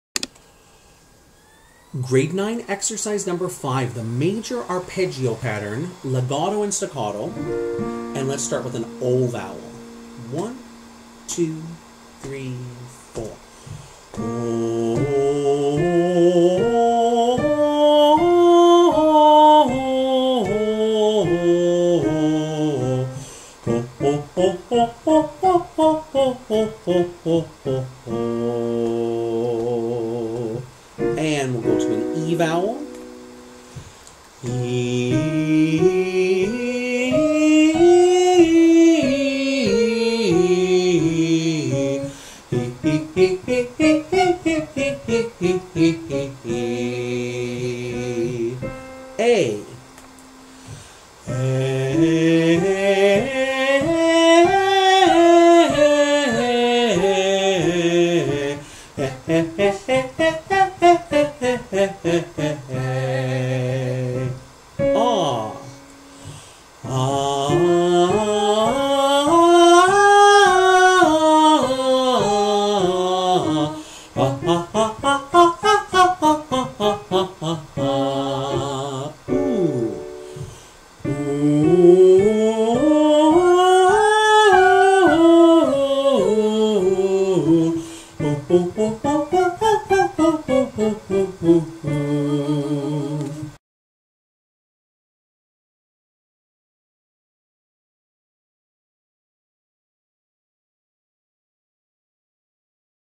Conservatory Canada Grade 9 Classical Voice Exercise 5
Recorded by Michael Faulkner in Belleville, Ontario, Canada on December 14, 2019.